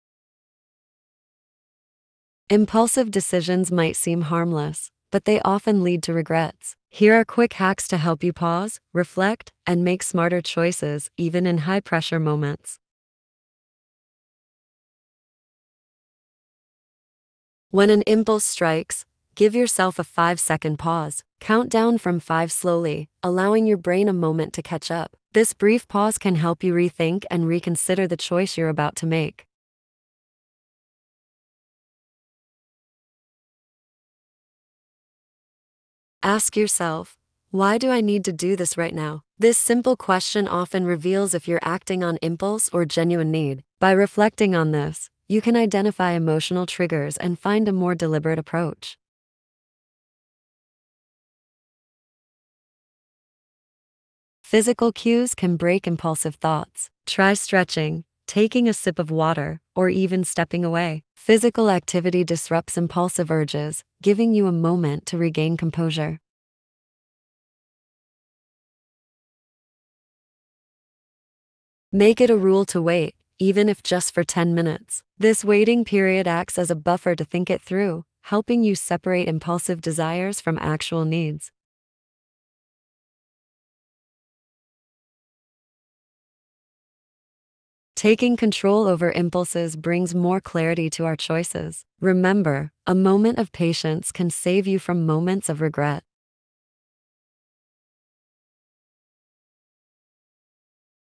Hacks to Stop Impulsive Decisions Instantly
Sometimes impulsive decisions lead to regrets. In this video, we’ll share quick, effective hacks to stop impulsive choices and make thoughtful ones instead.